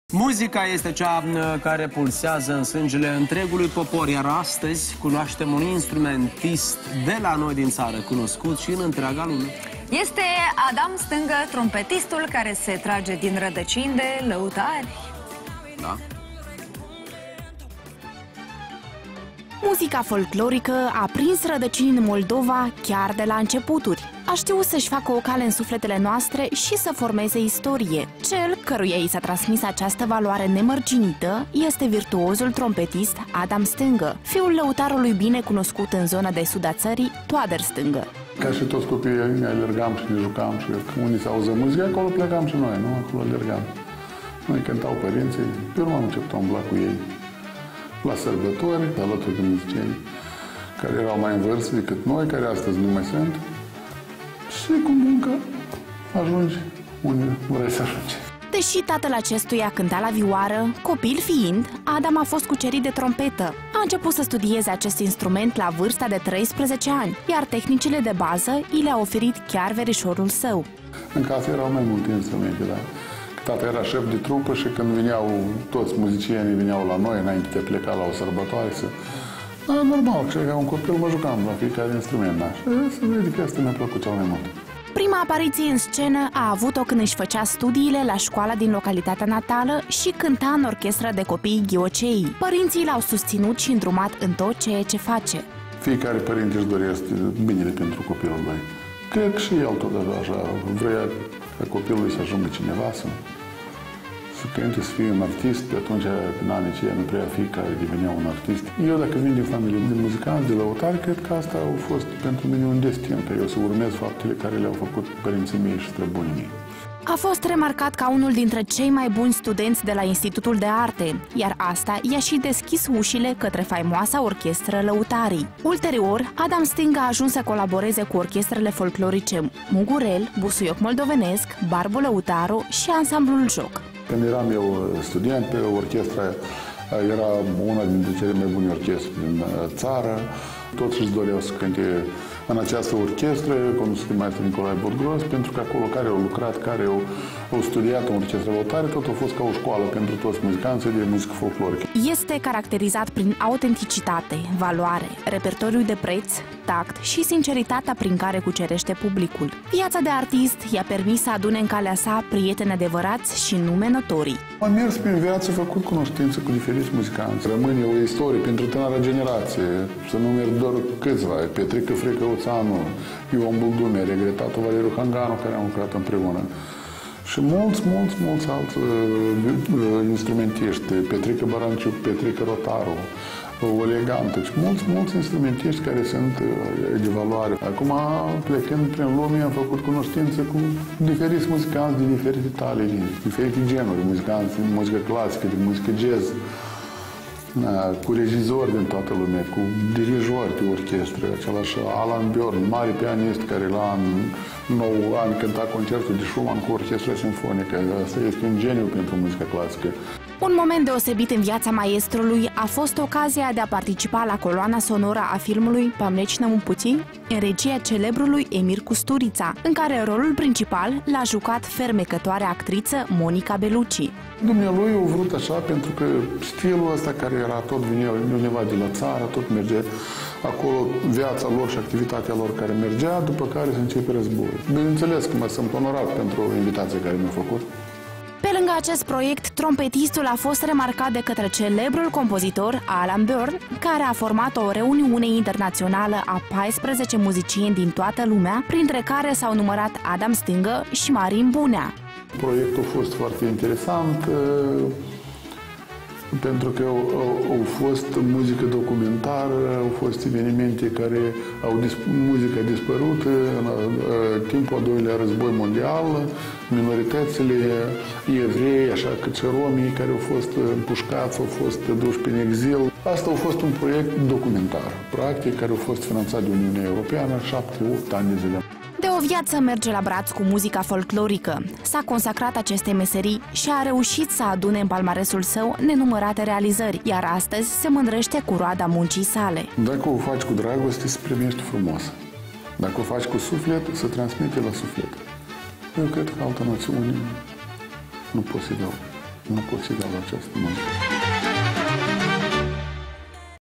Adam Stângă O VIAŢĂ ÎMBRĂŢIŞAT DE MUZICĂ
Muzica folclorică a prins rădăcini în Moldova chiar de la începuturi. A ştiut să-şi facă o cale în sufletele noastre şi să formeze istorie. Cel căruia i s-a transmis această valoare nemărginită este virtuozul trompetist Adam Stângă.